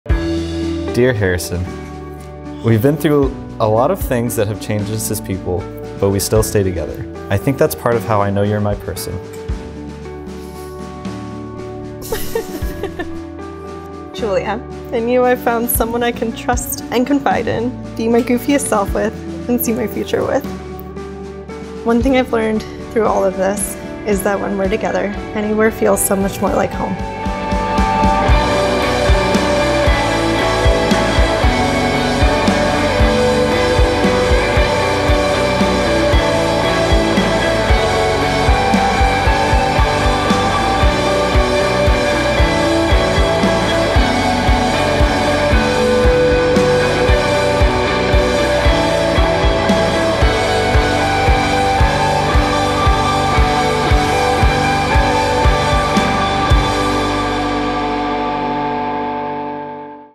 Julia & Harrison | Richmond, Virginia Ballroom Wedding | Wedding Teaser
“When we’re together, anywhere feels so much more like home.” 🥺

J+H spent their wedding day laughing in-between tears. Here’s a sneak peak of their big day!

Video: @venusfungfilms
Photo: @amelia_b_photography
Venue: @jmballrooms

Let's work together! Wedding film packages